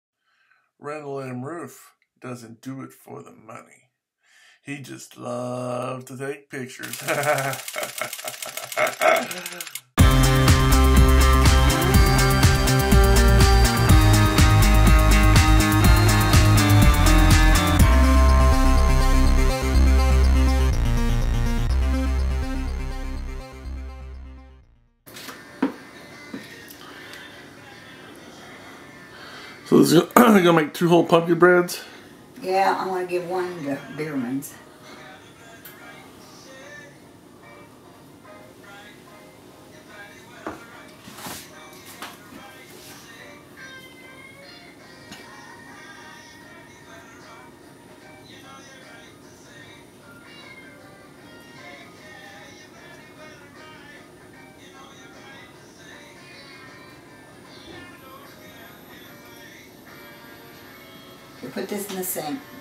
Donna Jean Ruff works on making some Pumpkin Bread on 9-10-2018 A.D.
Title : Donna Jean Ruff works on making some Pumpkin Bread on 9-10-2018 A.D.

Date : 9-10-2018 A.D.

Description : Donna Jean Ruff works on making some Pumpkin Bread on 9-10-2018 A.D.

Address : Randall M. Rueff
                  U.S.A.